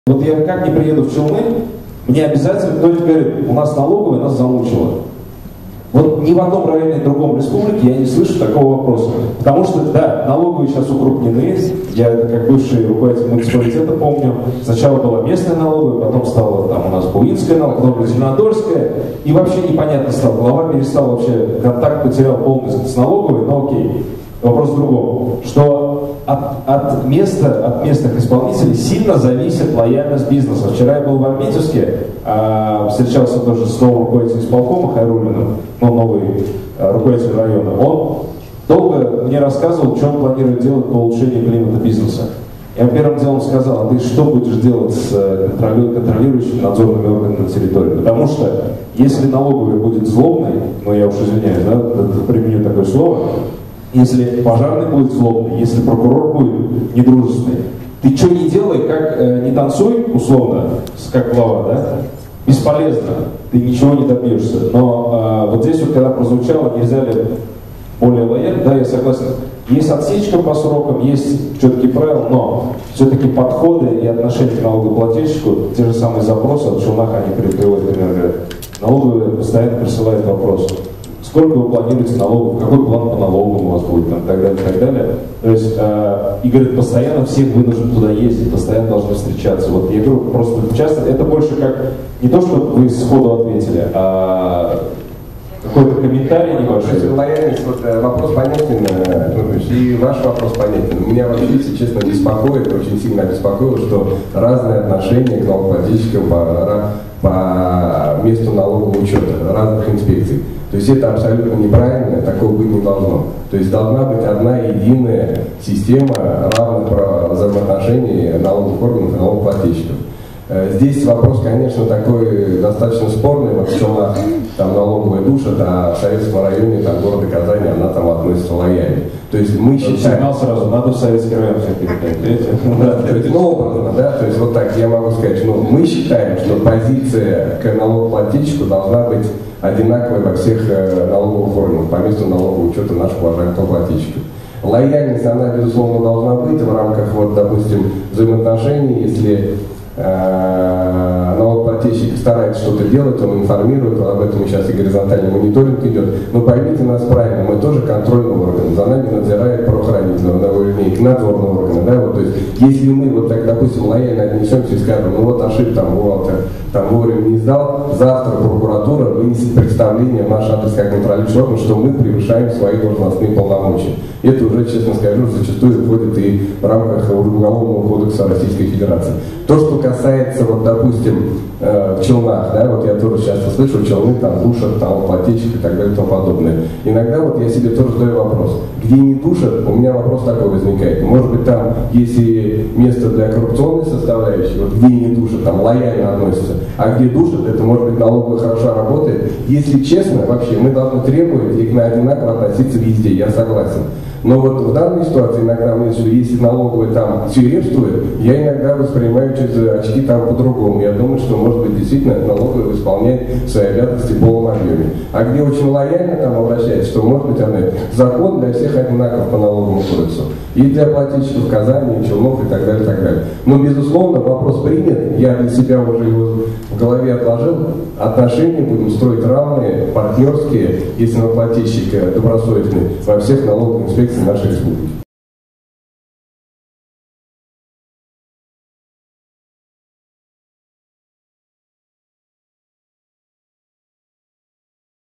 Марату Сафиуллину рассказали, как налоговая "душит" бизнес в Челнах
Руководитель УФНС по РТ Марат Сафиуллин на встрече с предпринимателями в рамках проекта «Бизнес и власть. Откровенный разговор» ответил на вопросы предпринимателей
Видео: Юлиана Семенова